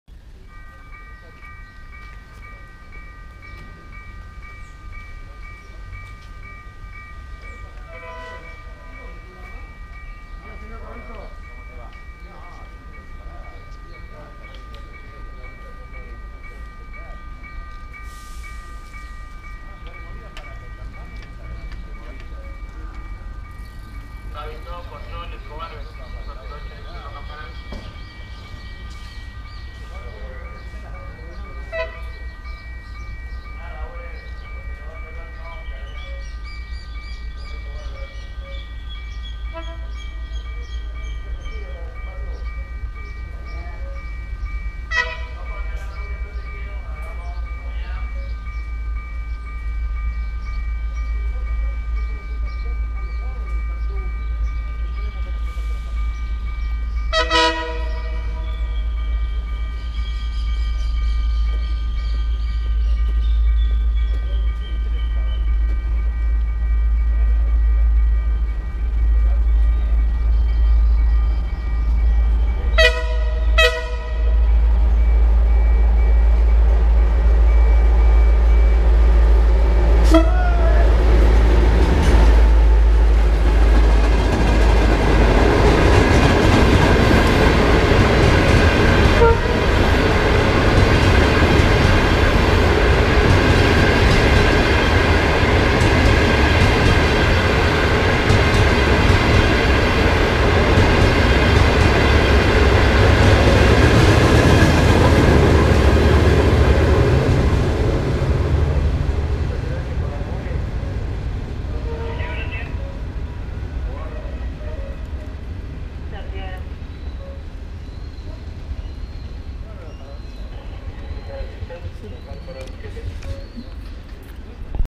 tren #278 El Rosarino en una mañana otoñal 🍁🍂🌧️ pasando por Escobar Rumbo a Retiro con🚂 CKD8H0007
Trenes Argentinos

tren Línea MITRE de larga distancia

pasada del servicio tren número 278 el Rosarino pasando por estación Escobar provincia de Buenos Aires.. en una mañana otoñal 🍁🍂 gris. húmedo

Rumbo a Retiro cuidad de Buenos Aires

procedente de Rosario Norte

Registro del vídeo: día sábado 6 de mayo del 2023 a las 07:55 horas